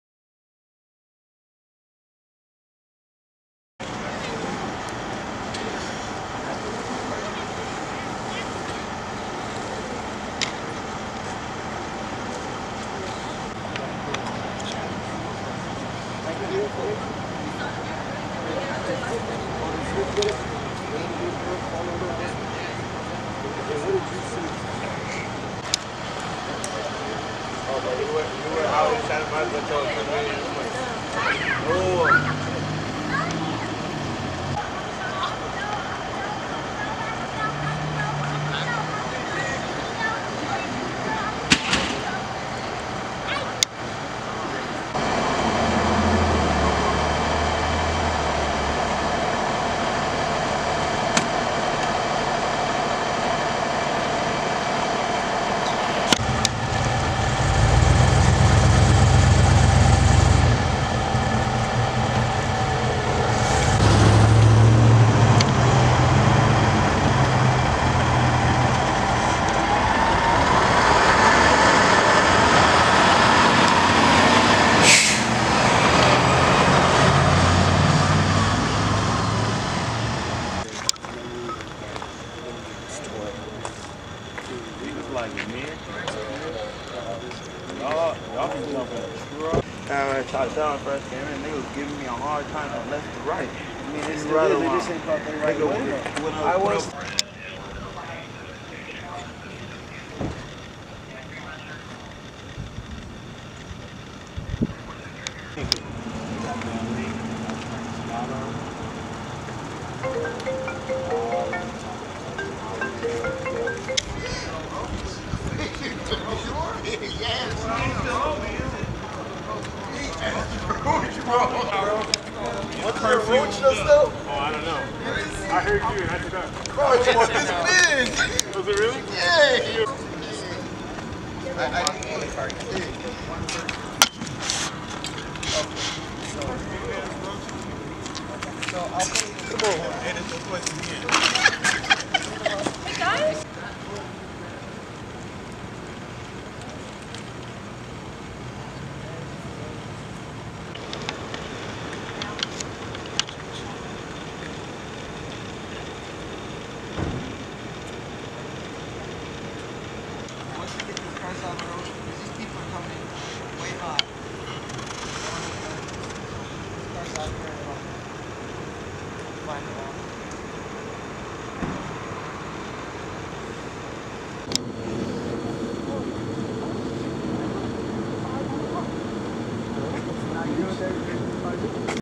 Hit and Run Trapped Crash / San Pedro 7.10.19
SAN PEDRO - One man was transported after being t-boned at an intersection. The suspect vehicle t-boned the victim vehicle at high speeds at the intersection, and an unknown amount of occupants fled on-foot, a witness stated off-camera. The driver of the victim vehicle was left trapped, and was extricated by firefighters and transported in unknown condition. A crime scene has been set up at the intersection.